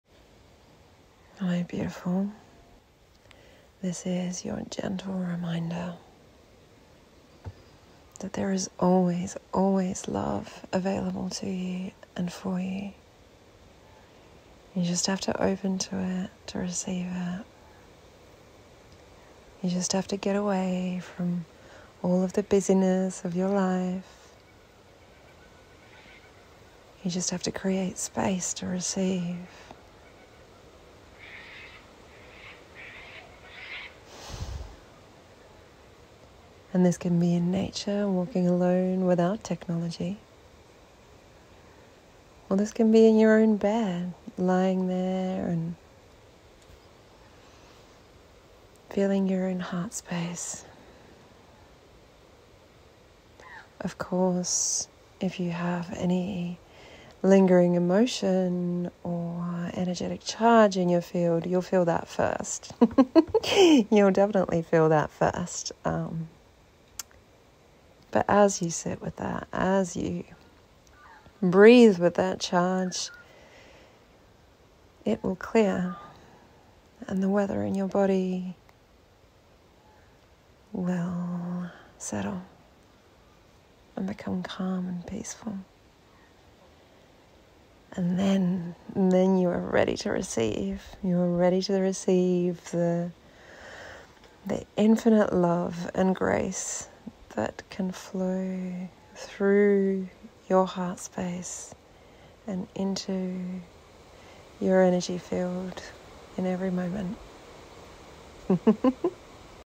There is so much love for you here
If you are feeling at all depleted, this message is for you.

Here is your permission to give loving attention to yourself. To rest within your own heart-space, and regenerate.

You deserve to be held and nurtured, as much as your loved ones. Remember to create space to receive your own love - especially after a period of heightened giving.